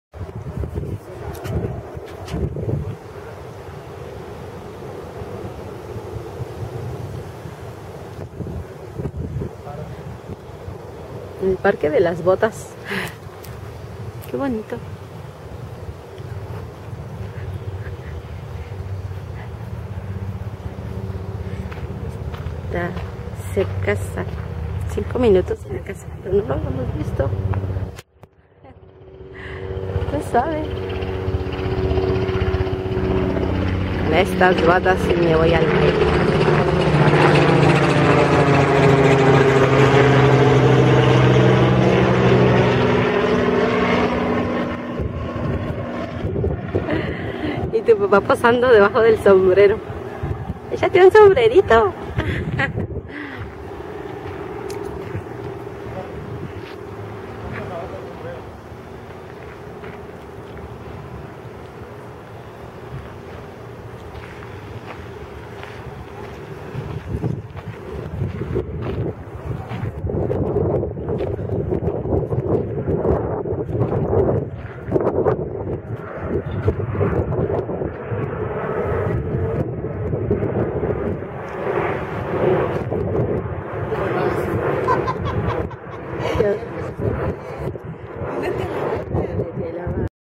Jardin de botasysombrero
En el parque,  con lily la bebe y mario.